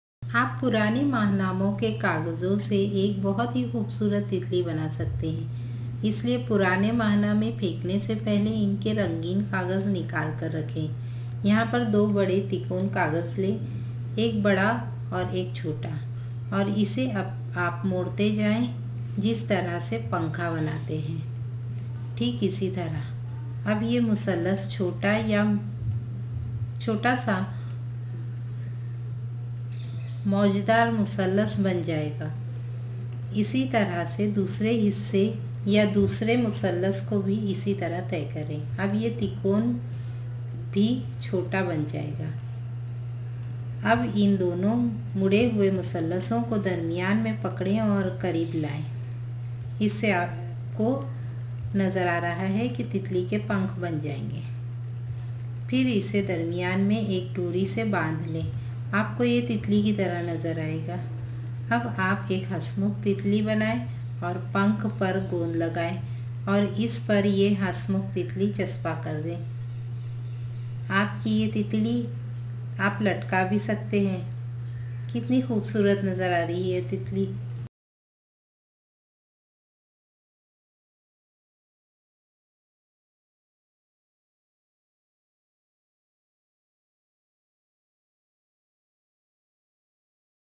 BEAUTIFUL BUTTERFLY | Urdu
You can make a very beautiful butterfly using colorful magazine paper. Cut two triangles one big and the other small from smooth, colored old magazines. Fold fan-fold pleats in the big triangle. This will crimp the paper and make the triangle corrugated. Do the same with the smaller triangle. Fold fan-fold pleats in the smaller triangle too. Then bring the two triangles together and they will look like the wings of a butterfly. Tie the two triangles in the middle with thread. You can see the front and the back view of the butterfly. Then with card sheet make the body of the butterfly with its antennae, head, eyes etc and glue it to the wings. The beautiful butterfly is all yours to admire. In the end you can hang it with a thread on the wall.

Dubbed in Malayalam by Sunil K Kolangara